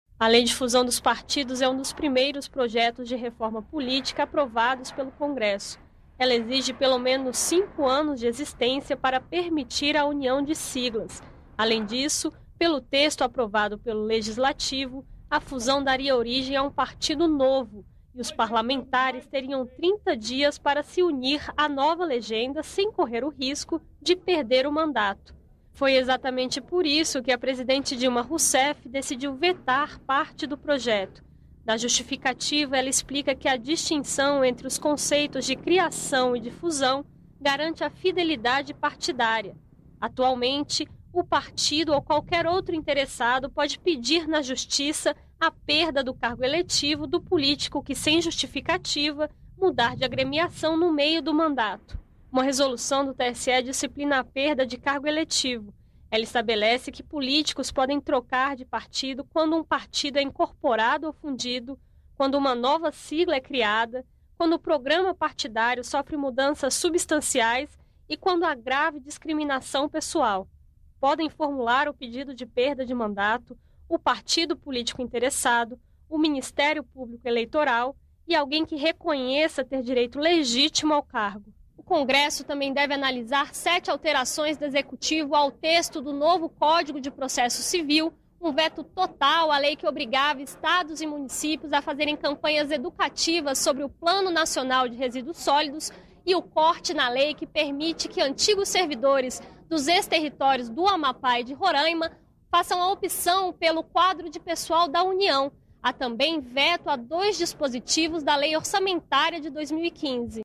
Veto ao projeto que impede a fusão de partido recém-criado está na pauta do Congresso
O Congresso reúne-se nesta terça-feira (16) para analisar vetos da presidente Dilma Rousseff. O veto ao projeto que impede a fusão de partidos políticos recém-criados está na pauta. Foi vetado dispositivo que estende às legendas formadas após fusão as mesmas garantias dadas aos novos partidos, que podem receber detentores de mandatos antes filiados a outros partidos, no prazo de 30 dias desde o registro, sem que isso acarrete perda do mandato desses novos integrantes.